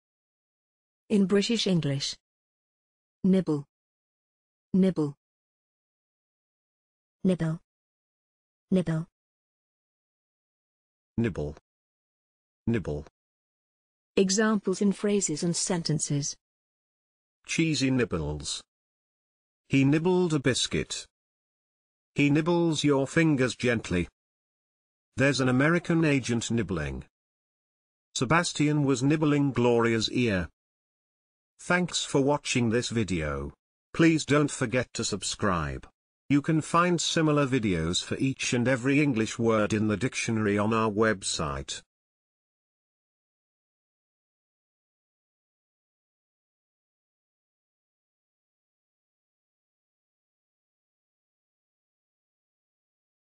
nibble - pronunciation + Examples in sentences and phrases
This is the pronunciation of nibble in British English + a few examples of its pronunciation in phrases and sentences.